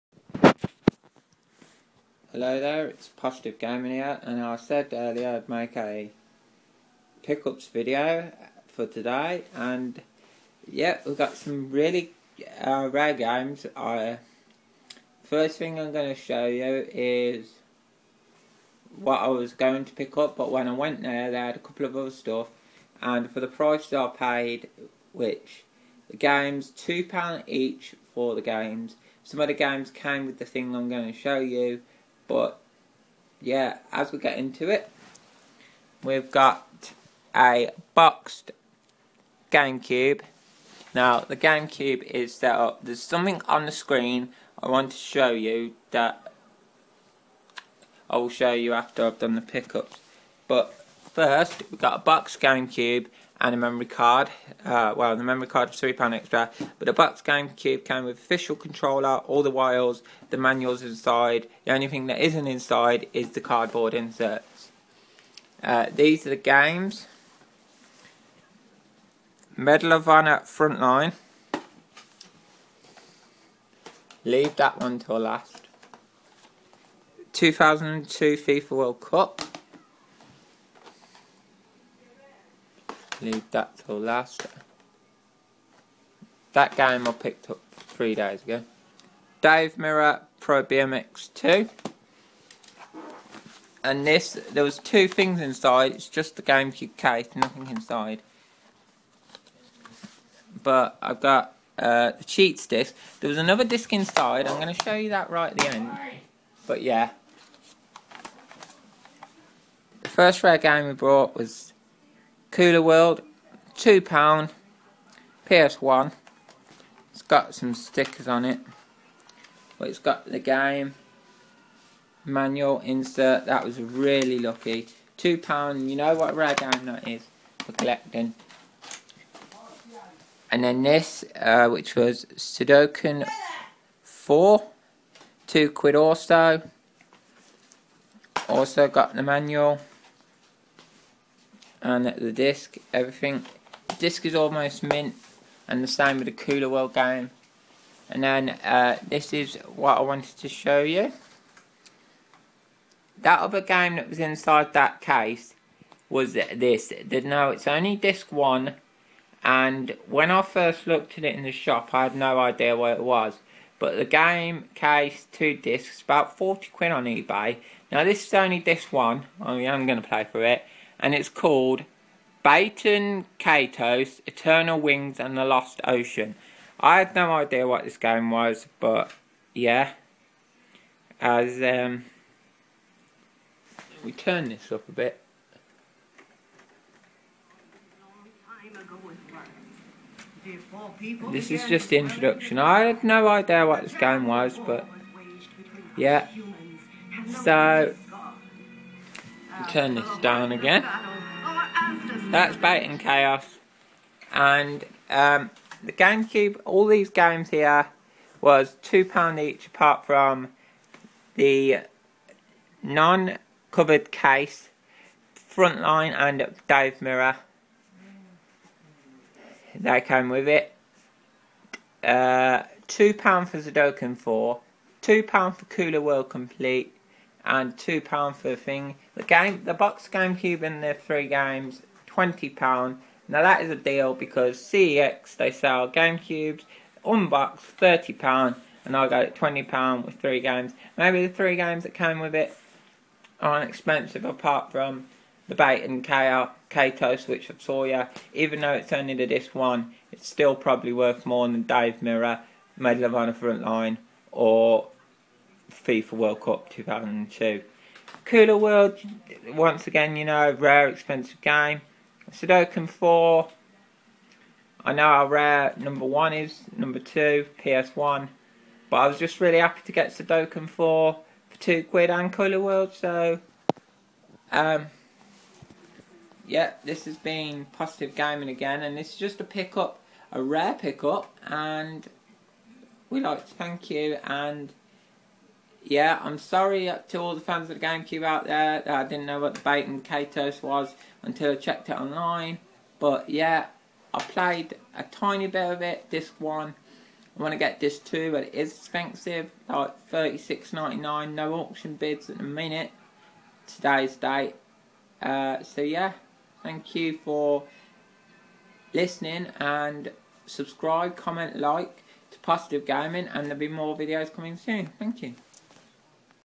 amazing pickups you wont believe the price
justa pick up video of my games and a console i gfot to day